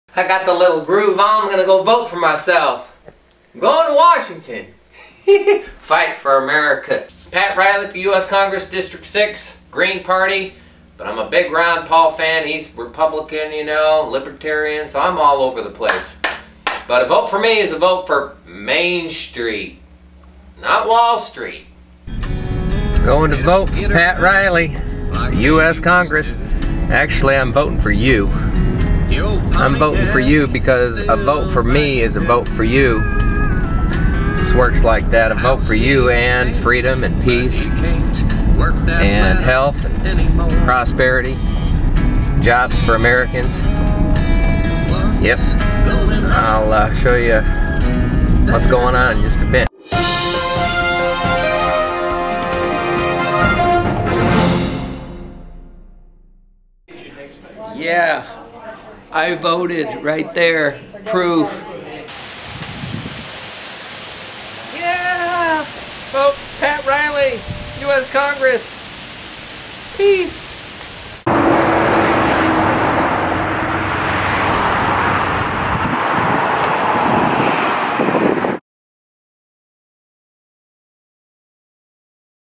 Pat Riley - Going to Vote - US CONGRESS
Documentary: Me going to vote for YOU. - Pat Riley running for US CONGRESS TN. District 6.